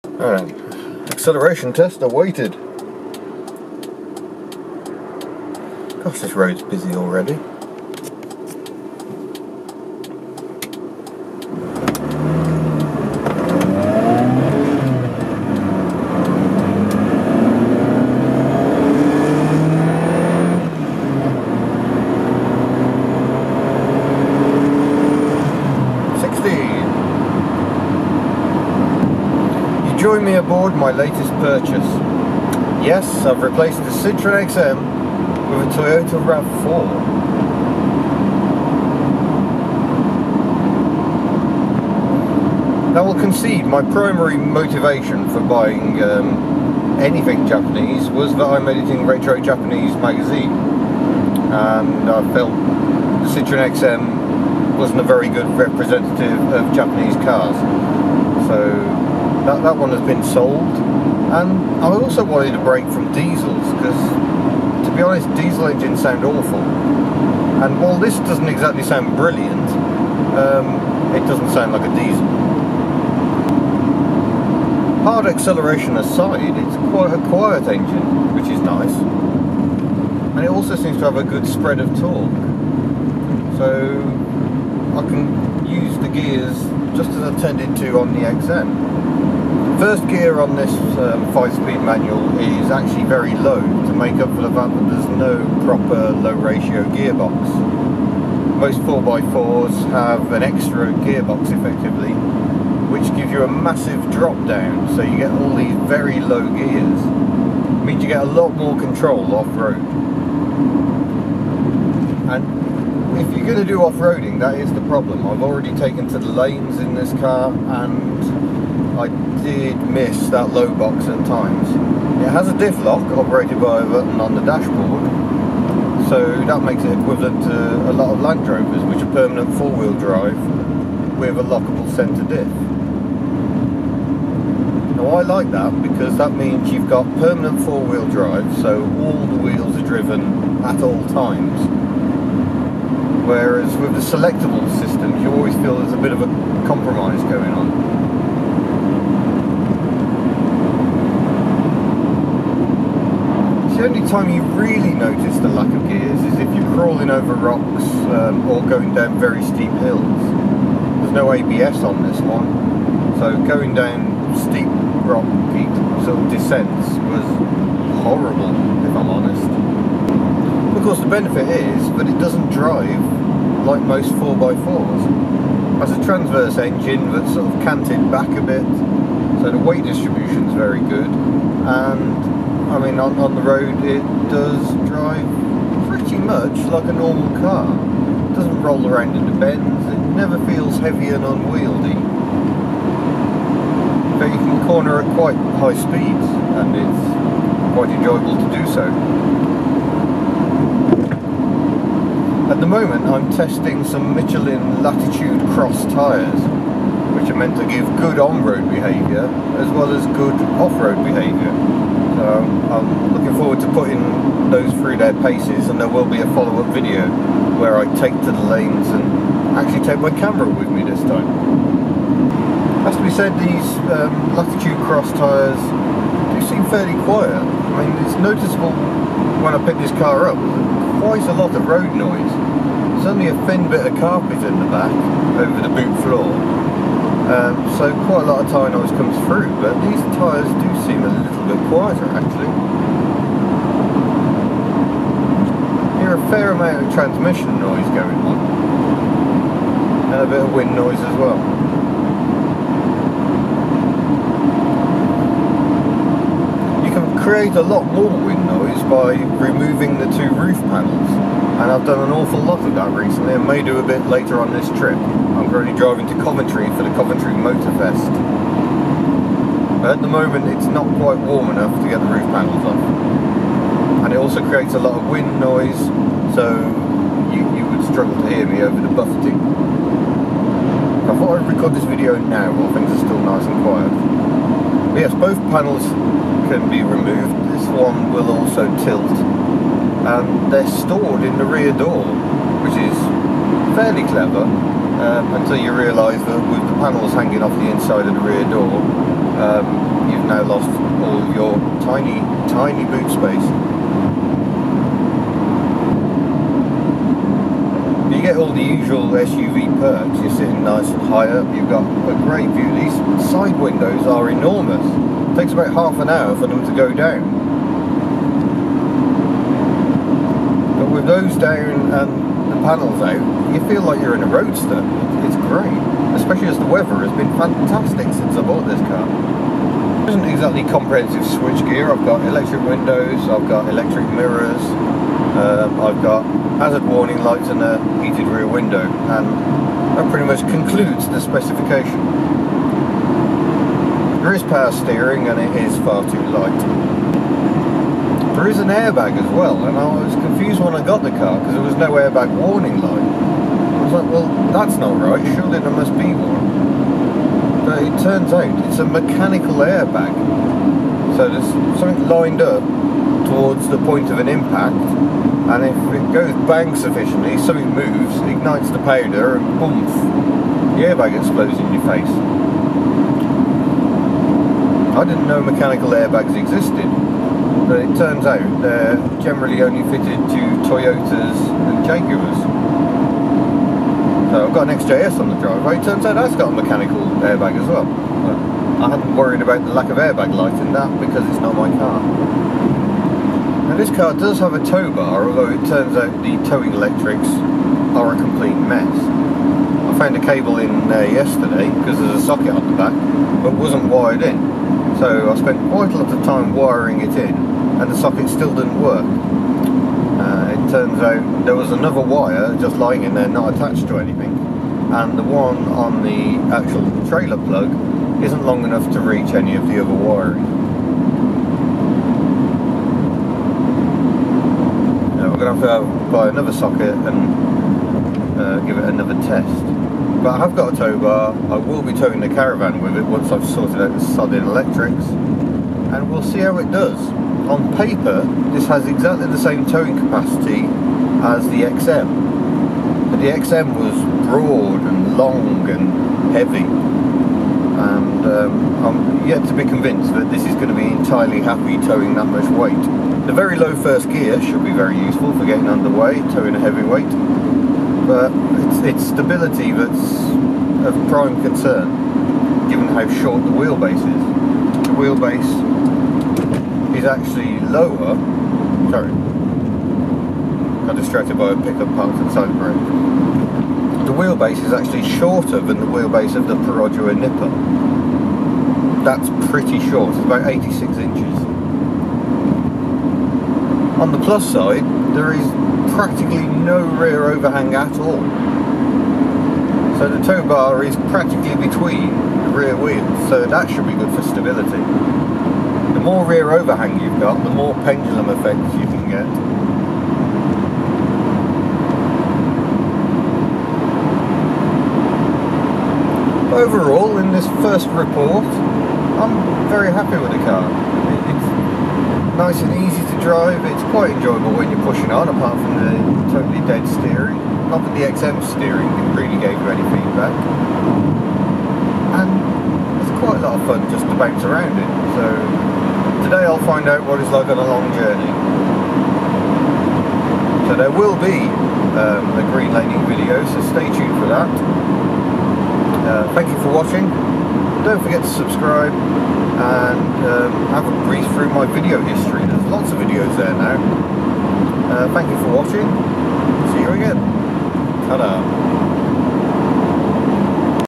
Road Test: Toyota RAV4 Mk1 - not as crap as you think!
Here's the video you've (not) been waiting for. First review of the car that has replaced my Citroen XM - a 1996 Toyota RAV4 GX 3dr.

Watch as I take you through the comprehensive dashboard. Be amazed by its power and acceleration. Contains a review of its greenlaning prowess. No, it actually has some!

At the risk of sounding posh, this was filmed on an actual camera rather than my phone. Normal service is catered for by the lack of script or any forward planning.